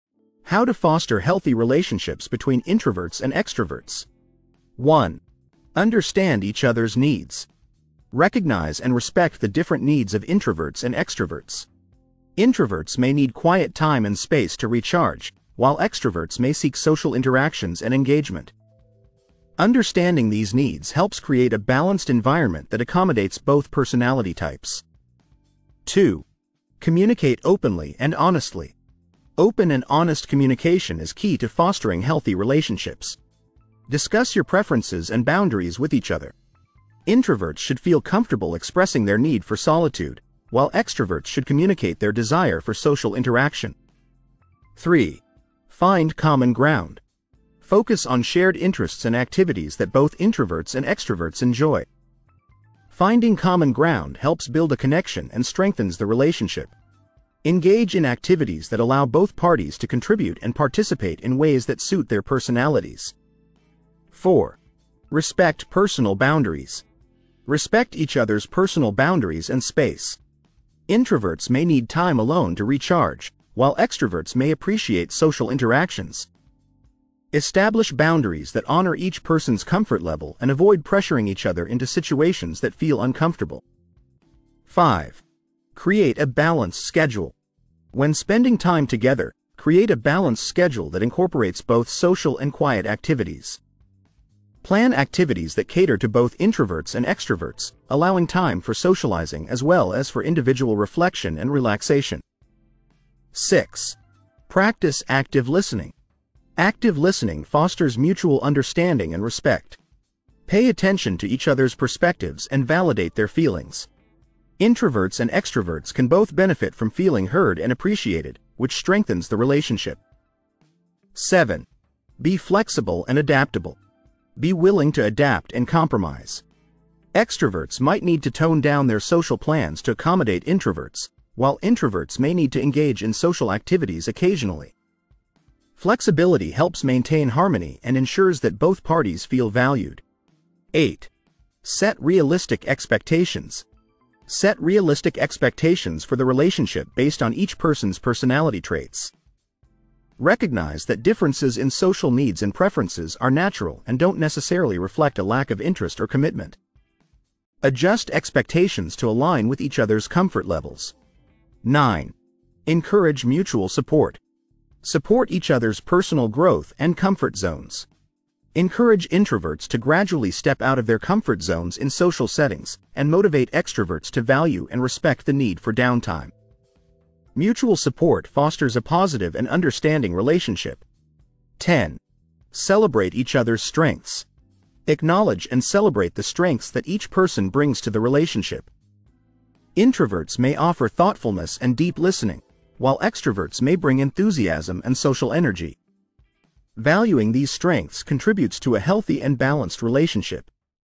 How to Foster Healthy Relationships Between Introverts and Extroverts
Competitive situations can be challenging. Learn how to navigate them effectively as an extrovert, using your social skills and high energy levels to achieve success while maintaining positive relationships.